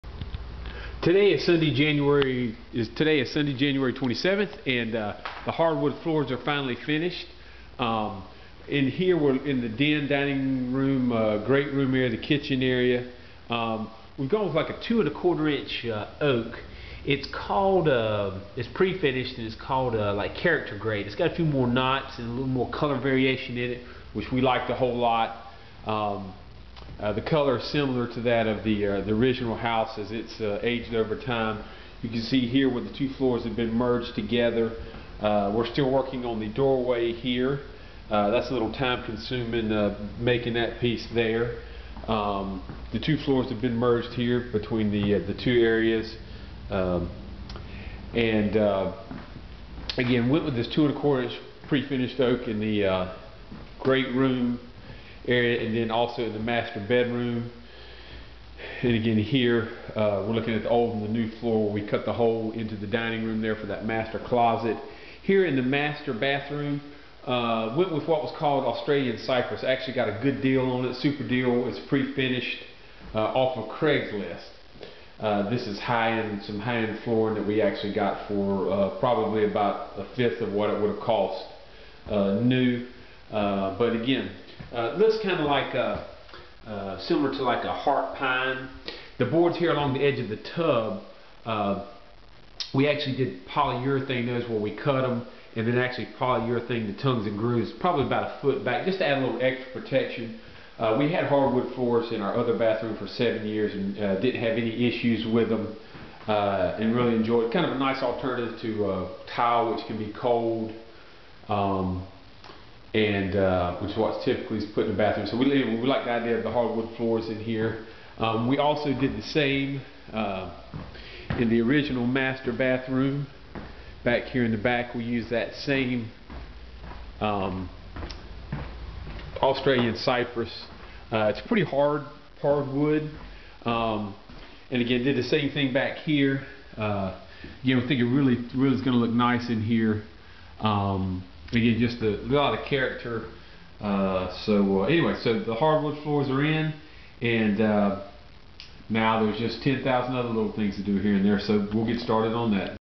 6 days down on our knees......
After 6 days down on our hands and knees, the hardwood floors are finally finished. Definitely took longer than I had hoped, but we could not have put it down any faster. Installed nearly 1000 square feet overall and it looks great. Some fancy new knee pads and a bottle of ibuprofen helped get us through the week.

Put down 2-1/4" prefinished oak in the master bedroom and greatroom. It's what some call character grade which means it got more knots and color variations. It really looks great. We bought it from Jess Long of Southeastern Wood Products. If you're in the market for hardwood floors I'd highly recommend giving Jess a call.

We installed 5" wide Australian Cypress in the new and original master baths.. It is absolutely beautiful. Got an awesome deal on it off of craigs list.

It was a long week with a lot of late nights, but we got it done. Thanks Dale for all your hard work!